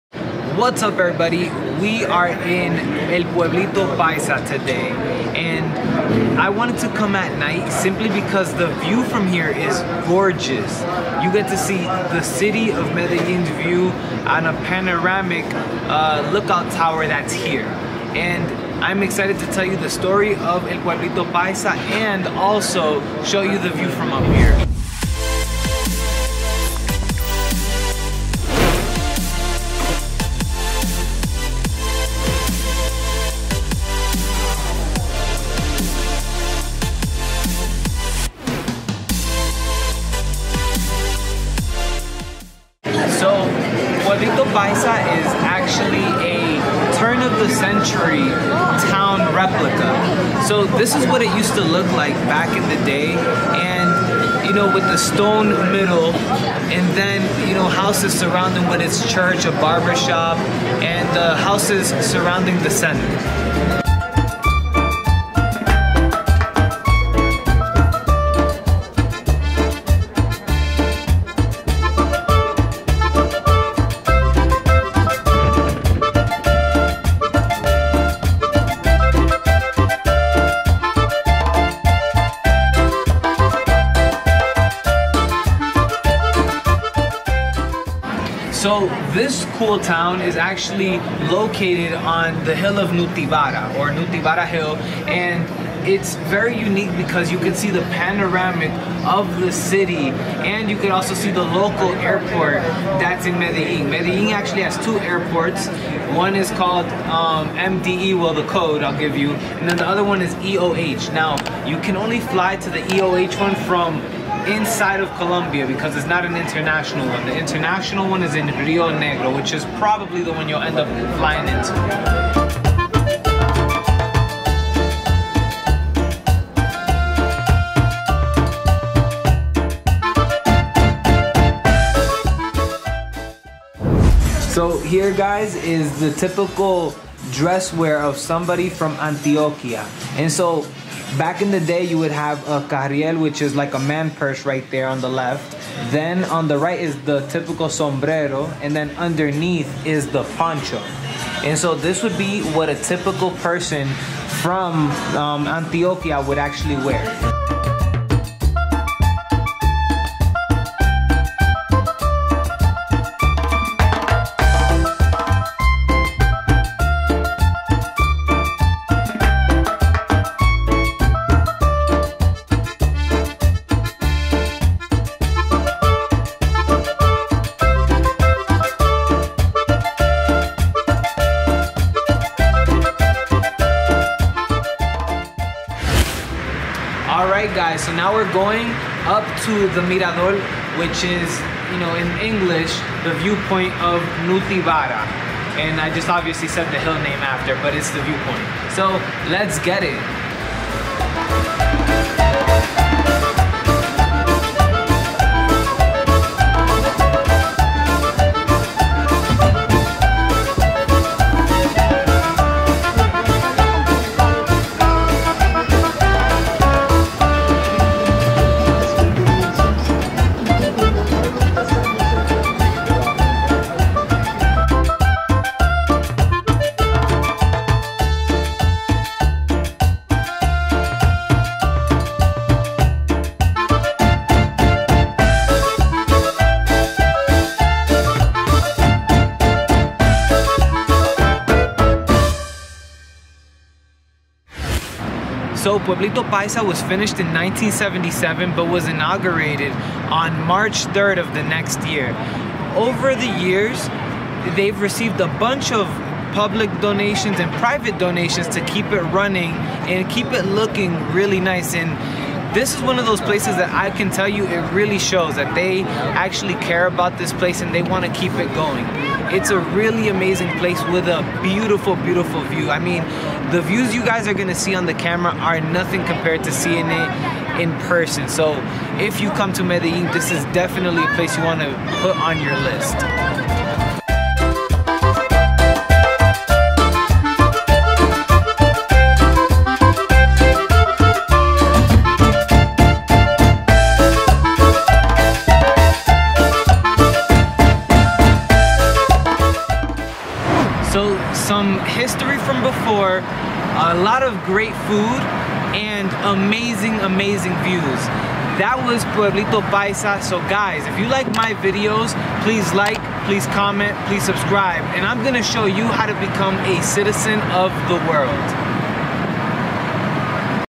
Pueblito Paisa - The place with the best views of Medellin
Pueblito Paisa is located at the top of Cerro Nutibara (Nutibara Hill) and it’s one of the most popular tourist attractions in Medellín.

This little town is a replica of a turn of the century town in Antioquia, complete with a fountain in the middle of a cobblestone town square plus a white church. It  offers great views of the city of Medellín and the surrounding mountains. it was closed for covid, but now it reopens it's doors for everyone!

See you citizens!

If you want to learn my travel algorithm to plan your trips and fly at the best price go check out my course